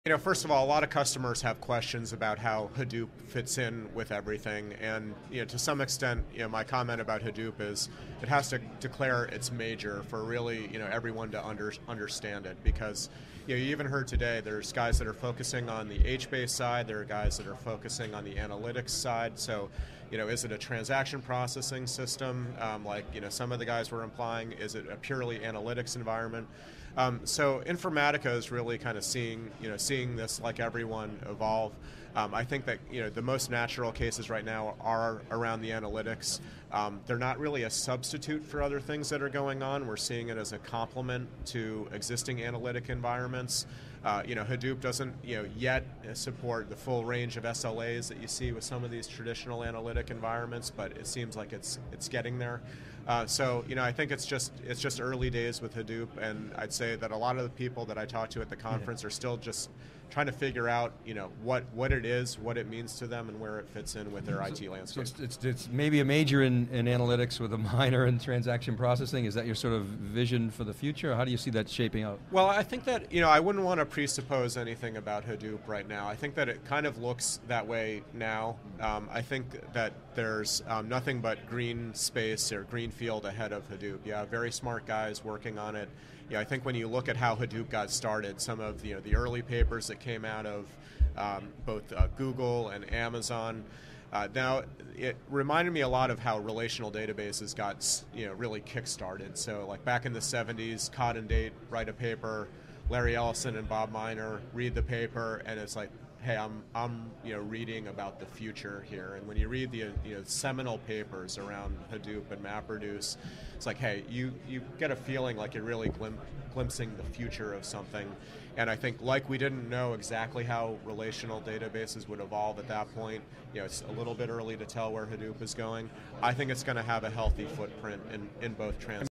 Informatica's Markarian: Hadoop Has Yet to Declare its Major
Is Hadoop largely an analytic environment or a transactional environment? Informatica's James Markarian says Informatica is helping its customer navigate the early days of Hadoop while the open source framework finds its identity.